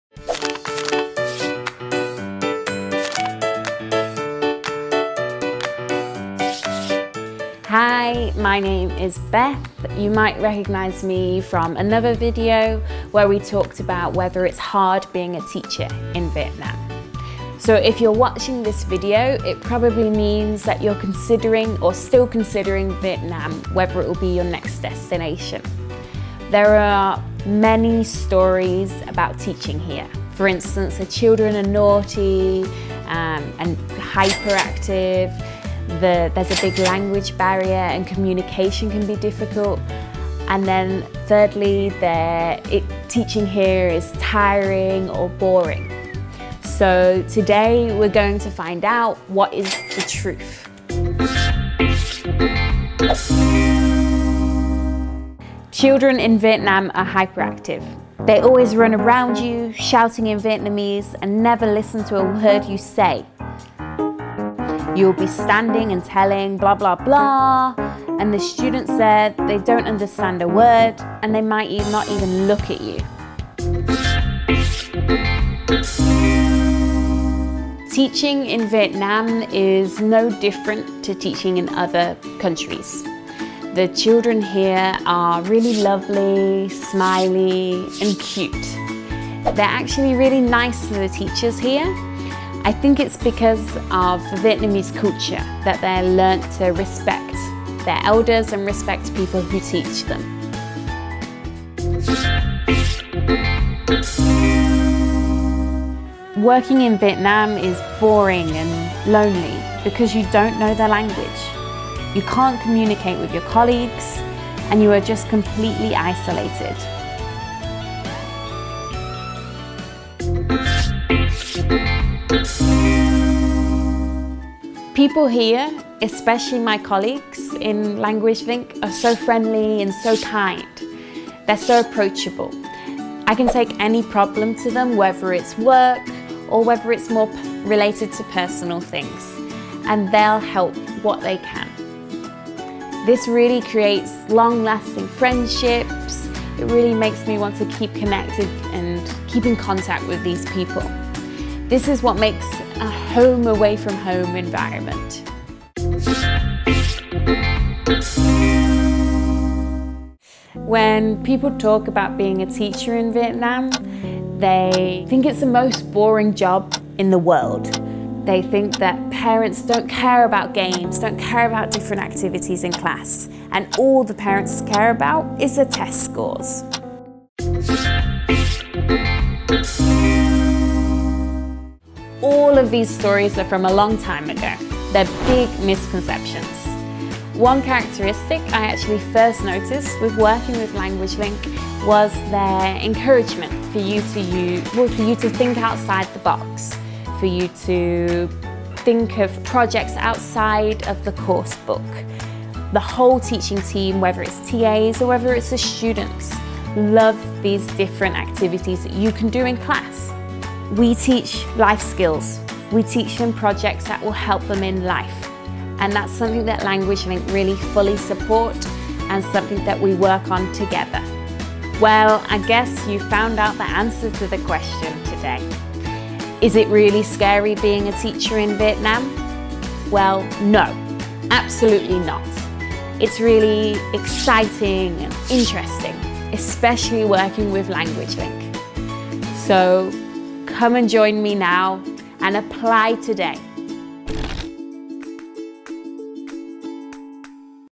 Teach at Language Link: Is it scary to be a teacher in Vietnam?
Listen to Beth's story and find out why you should work at Language Link Vietnam.

Language Link Vietnam - Học ở đây. Graduate anywhere.